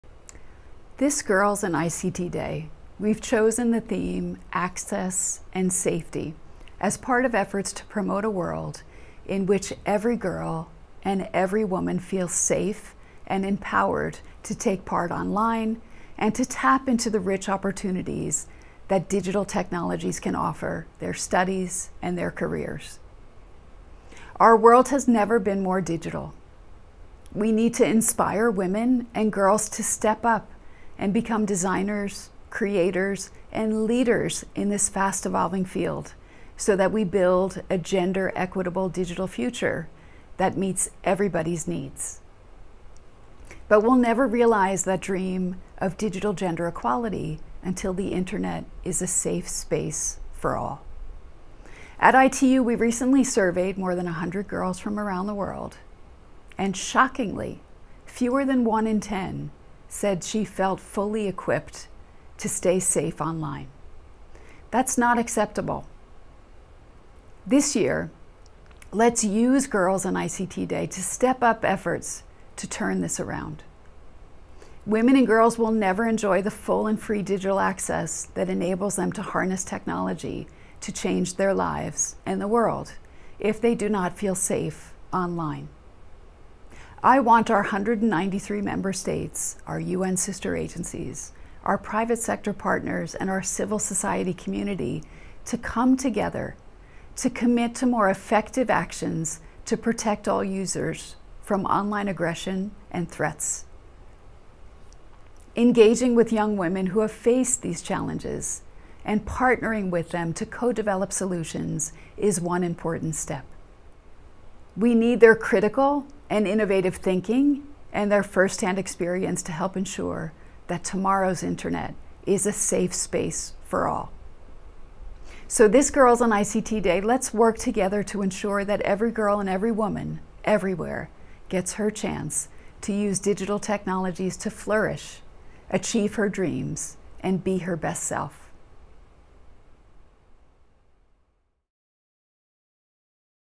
Girls in ICT Day 2022 - Video Message from the ITU BDT Director
Video message from Doreen Bogdan-Martin, Director, Telecommunication Development Bureau (BDT), ITU for International Girls in ICT Day 2022